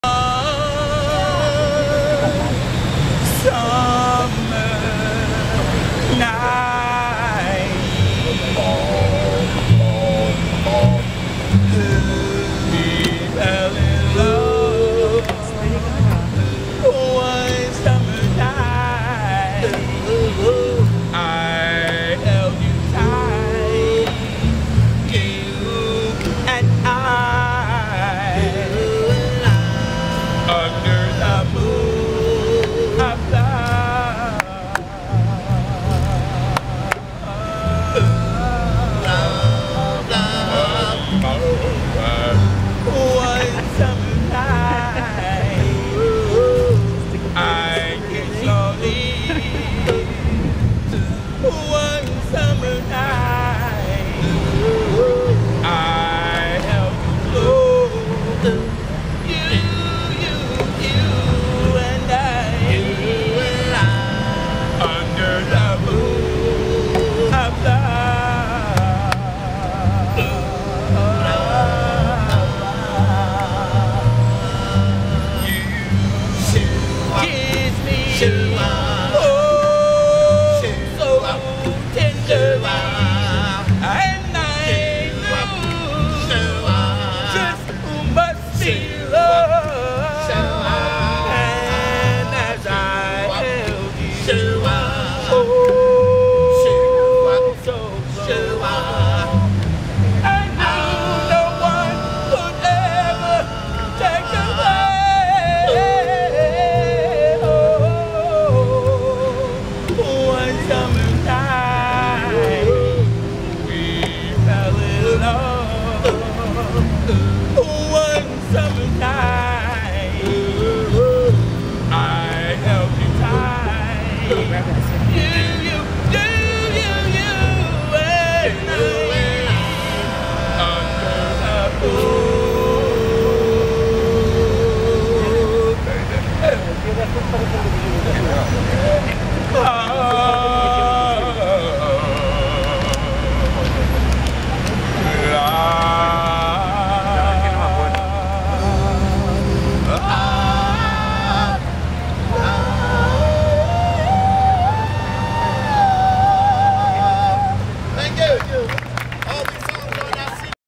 Acapella soul
We met these guys outside the Metropolitan Museum of Art in NYC on Thursday.  There was a lot of great street music in NY this week!